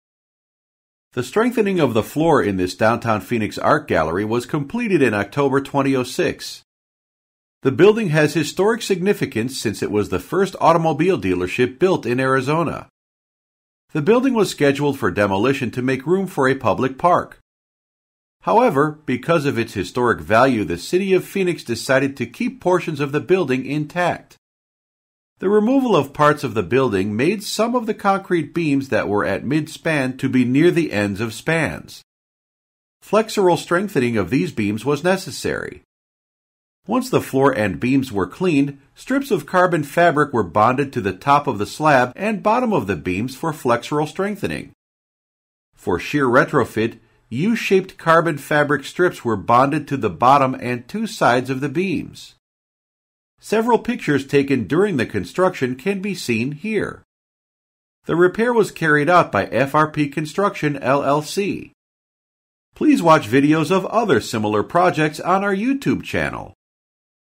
Strengthening of Concrete Floor Slab and Beams in Historic Building in Phoenix with Carbon FRP
A historic building was being converted to an art gallery.  The slab  and beams were strengthened with carbon FRP fabric for increased flexural and shear capacity.